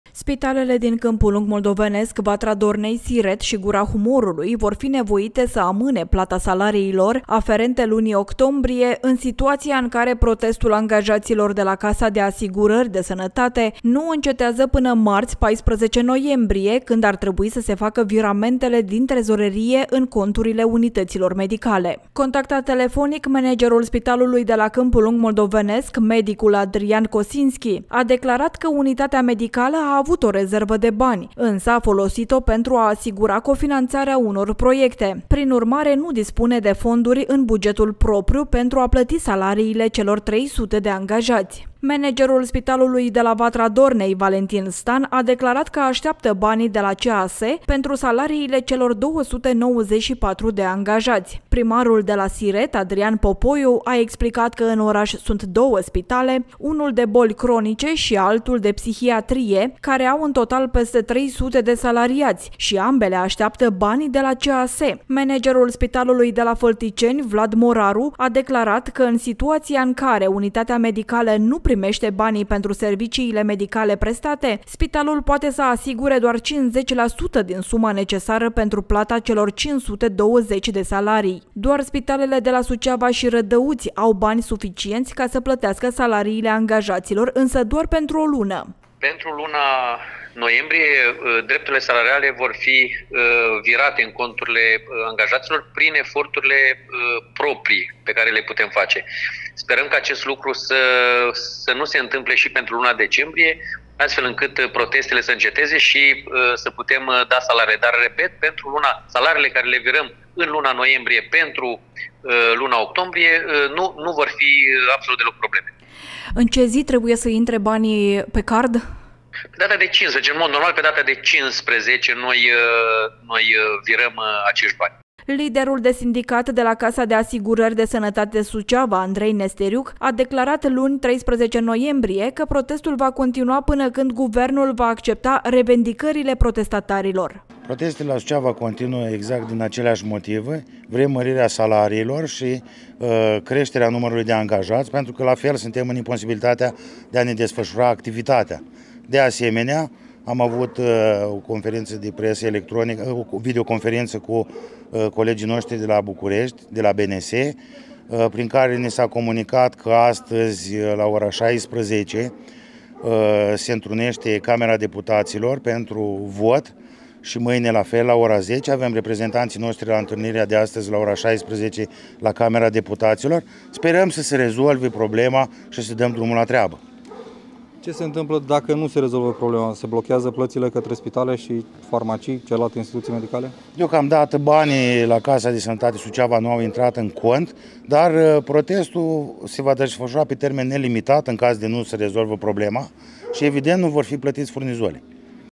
Aproape toate spitalele din județ, dependente de banii de la CAS
Protestul angajaților de la Casa de Asigurări de Sănătate Suceava scoate la lumină situația financiară precară a spitalelor teritoriale. Spitale precum Vatra Dornei, Câmpulung Moldovenesc, Gura Humorului și Siret sunt lună de lună dependente de banii virați de Casa de Sănătate inclusiv pentru salariile angajaților. Doar două spitale din județ au fonduri suficiente în buget ca să acopere salariile pe o lună.